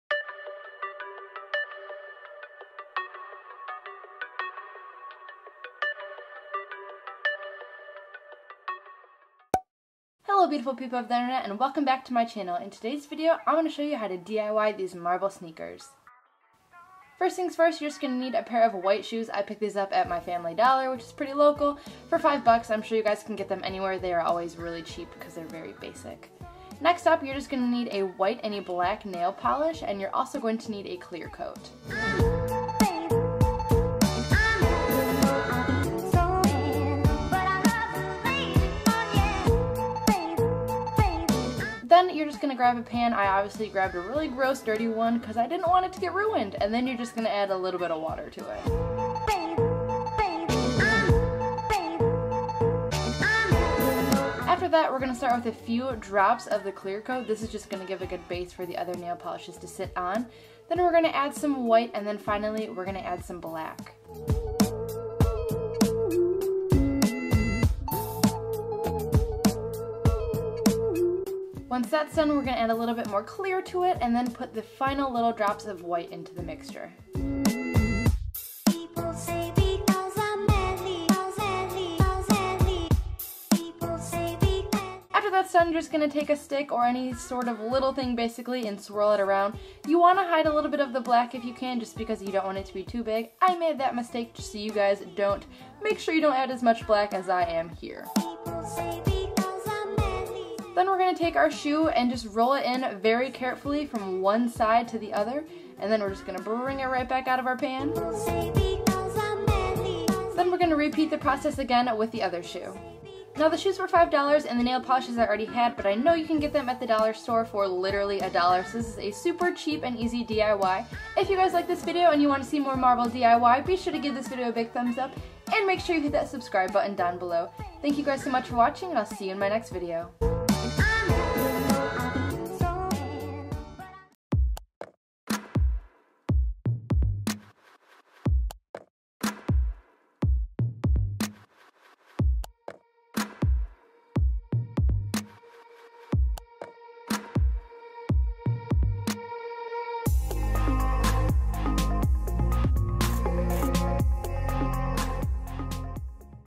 DIY MARBLE SNEAKERS ($5 BACK TO SCHOOL DIY)
What's better than a $5 DIY? One you can wear everyday to school!
In today's video I show you guys how to DIY marble sneakers for $5. You'll never guess what items you'll need to make these bad boys!